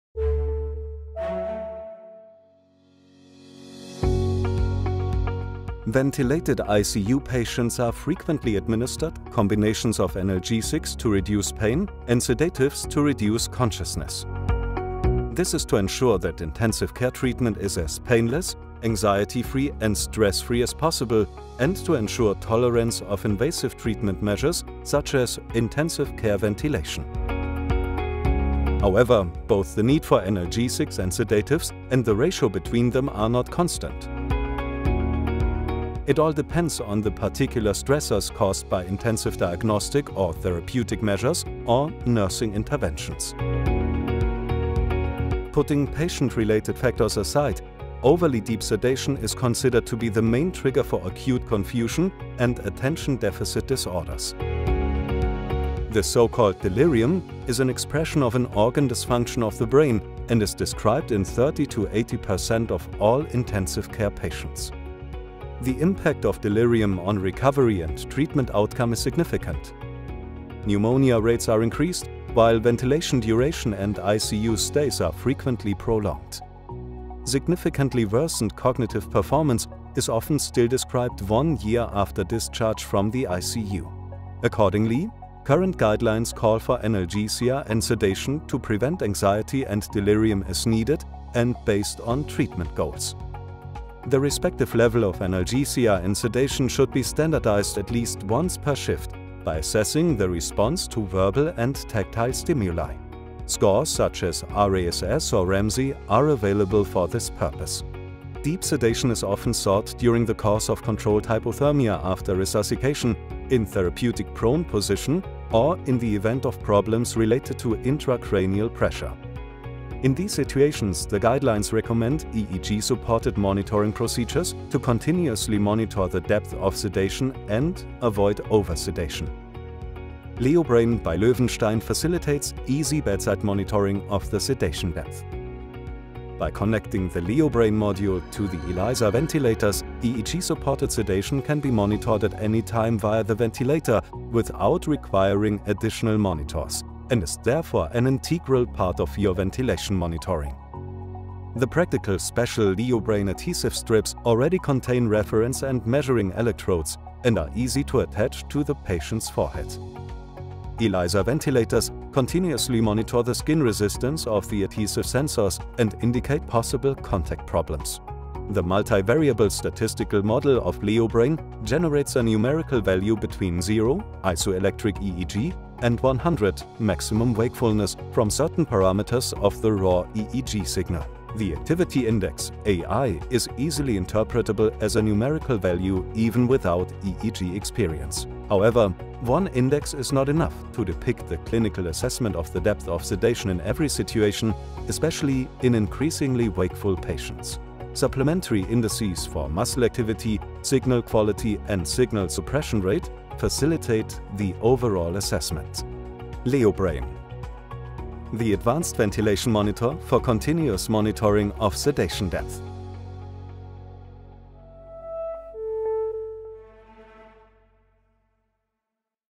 LEOBRAIN – Guideline-compliant sedation even in difficult situations
Overly deep sedation of ventilated patients is associated with a significantly longer ventilation period, increased incidence of pneumonia, higher incidence of delirium and increased length of hospitalisation. It is not always possible to determine the depth of sedation using scores such as the Richmond Agitation-Sedation Scale.
LEOBRAIN permits continuous monitoring of the sedation depth even in the therapeutic prone position, in relaxed patients and under conditions of controlled hypothermia following resuscitation.
Reduce the risk of delirium with LEOBRAIN EEG-based sedation monitoring when classic scores can no longer be used.